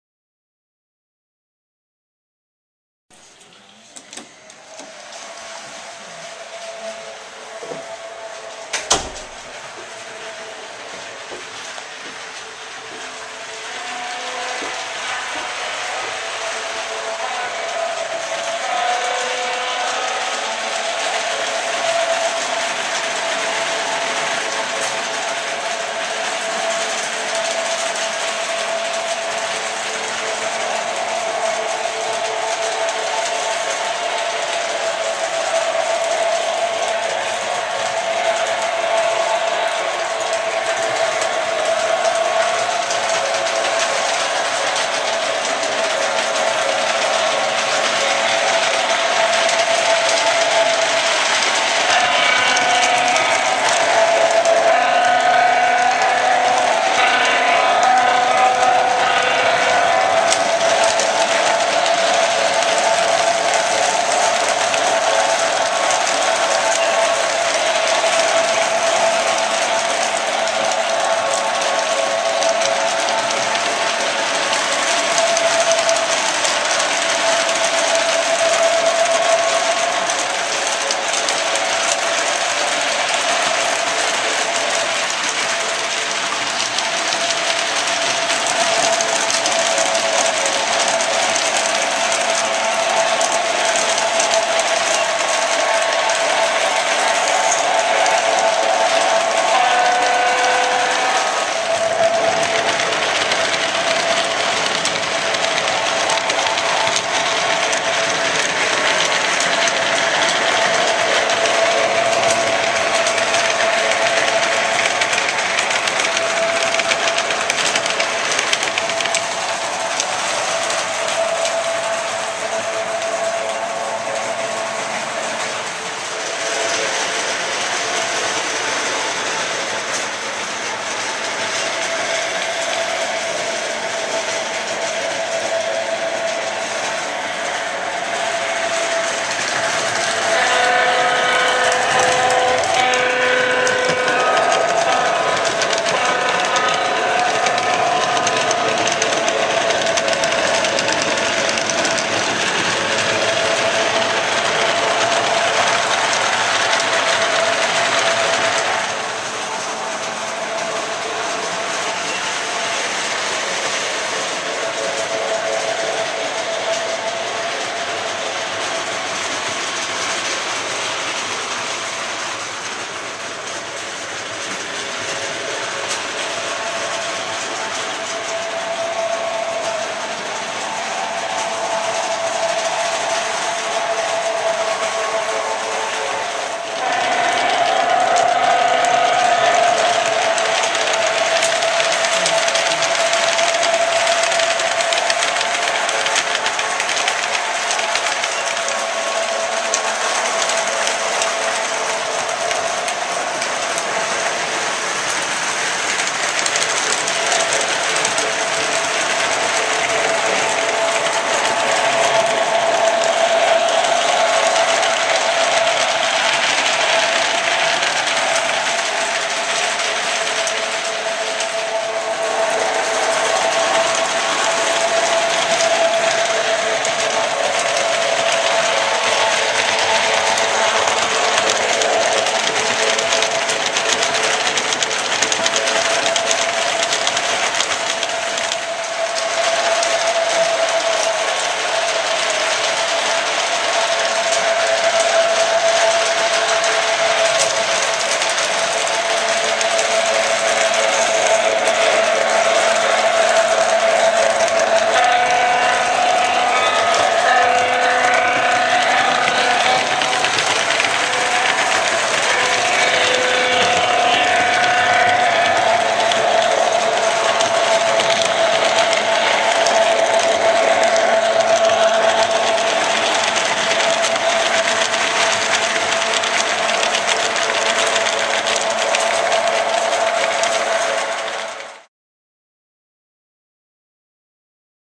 Dad's Lionel Layout - Update - 5/31/11
The first table is pretty much done. The mountain table is next!